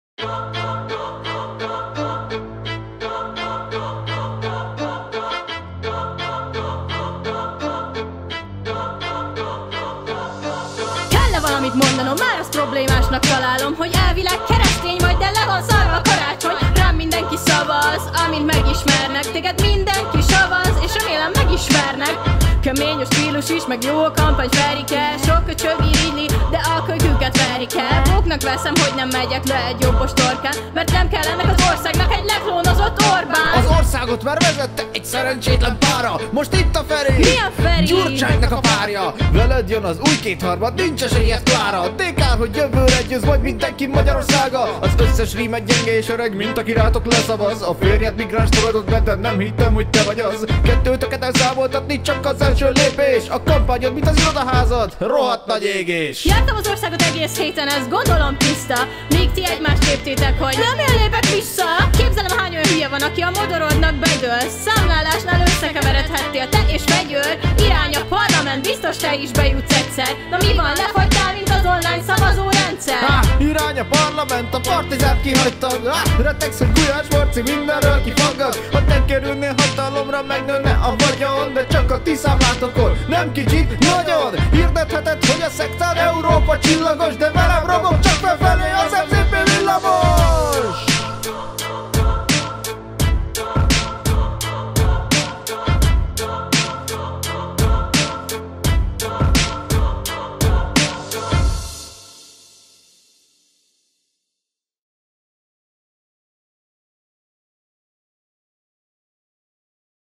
Márki-Zay Péter vs Dobrev Klára - Homokóra Mini #10
▼ LINKEK ▼

▼ SZEREPOSZTÁS ▼
Dobrev Klára:
Fox

Márki-Zay Péter:
Kenry26

▼ STÁBTAGOK ▼
Írta, vágta:
Kenry26

▼ PRODUKCIÓ ▼
Kamera:
Nikon D3400

Alapötletet nyújtották:
Nice Peter és EpicLloyd